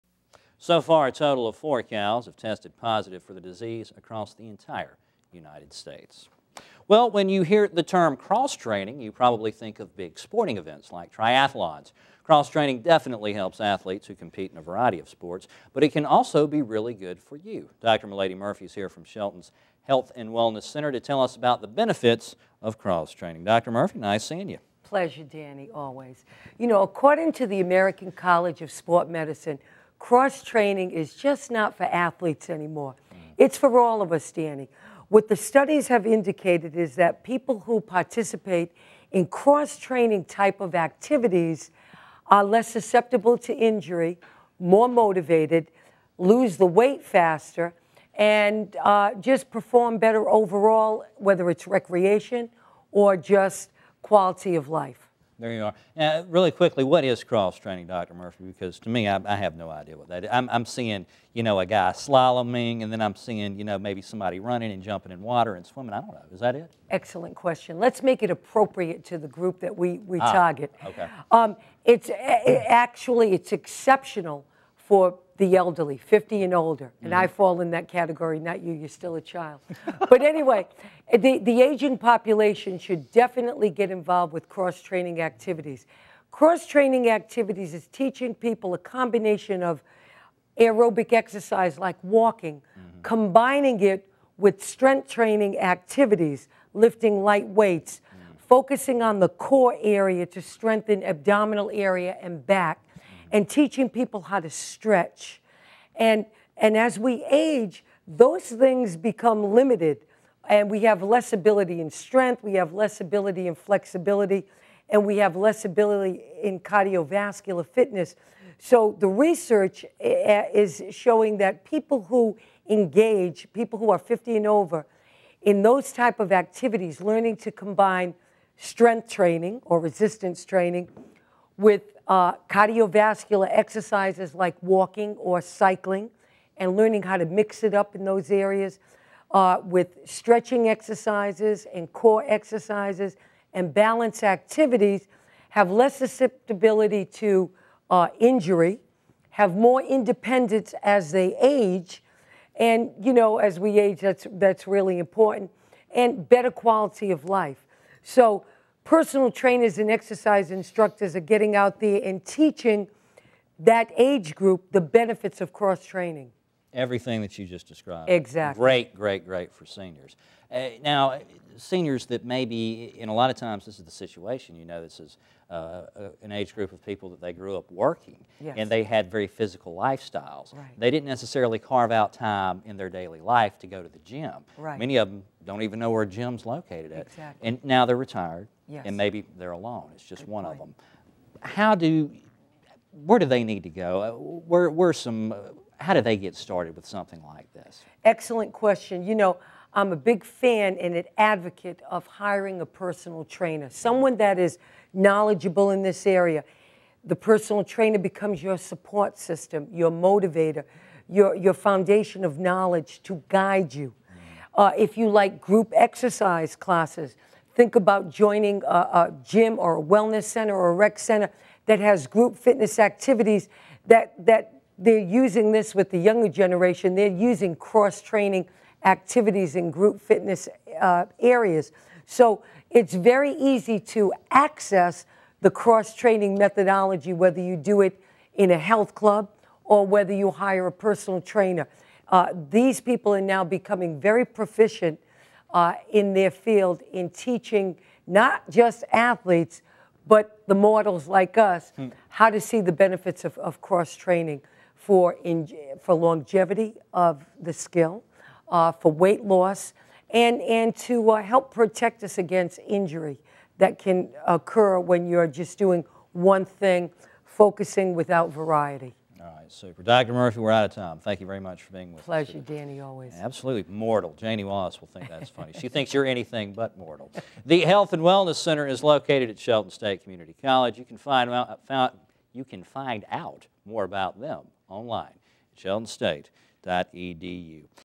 Milady Murphy Cross Training
When you think of cross training you probably think of versatile athletes competing in grueling marathons. But, cross training can be very beneficial for everyone, especially the elderly.

OAD: Wednesday, April 25th, 2012